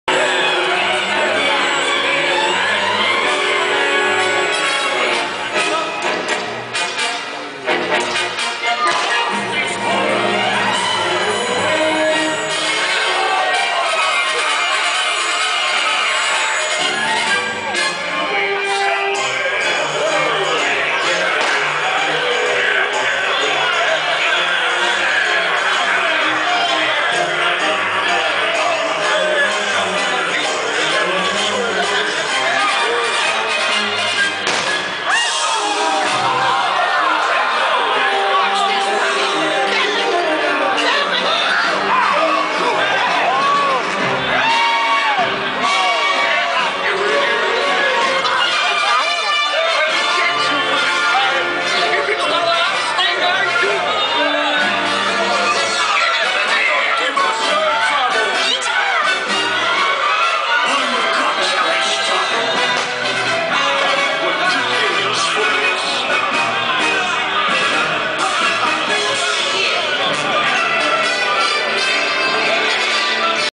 Disneyland's Fantasmic Pirate Ship
This is the Pirate Ship Columbia where you see the fight between Peter Pan and Captain Hook during Fantasmic.  This was shot in 2009.